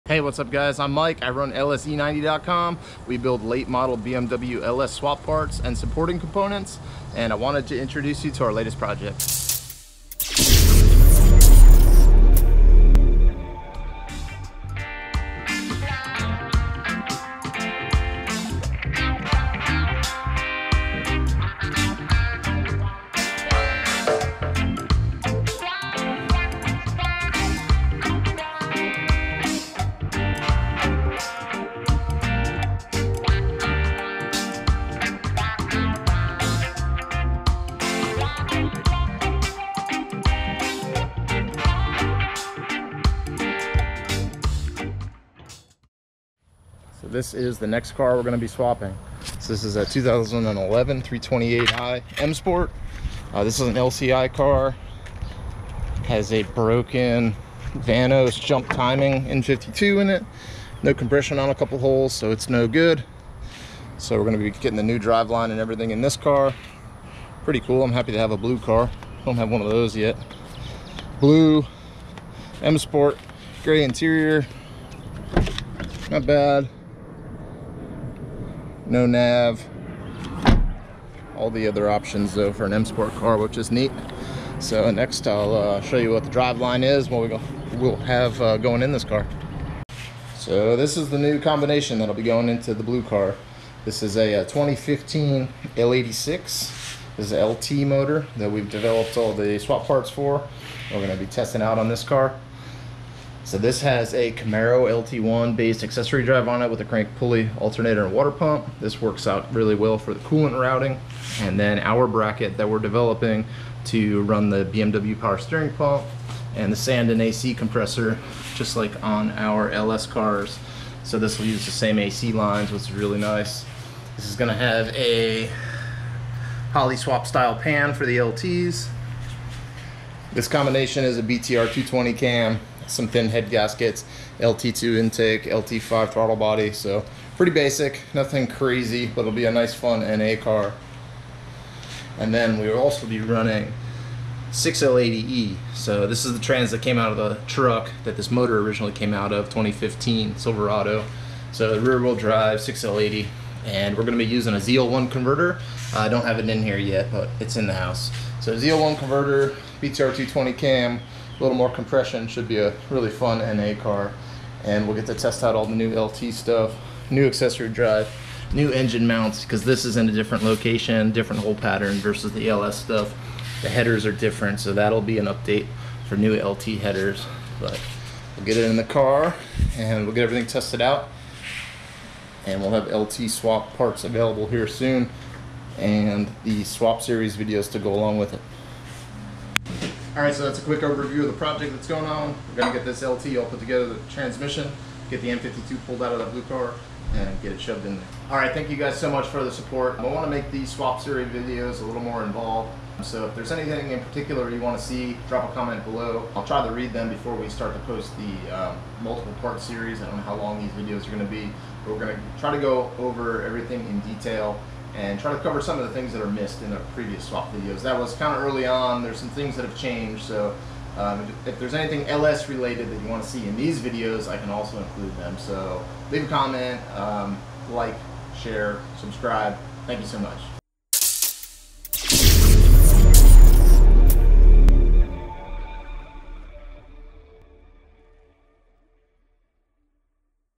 LSe9x LT Swap Series Introduction
Quick introduction video to Lse90s LT swap swap series. We will be dropping out the N52 in this 2011 BWM E90 and repowering it with a 6.2l LT and 6l80e powerplant.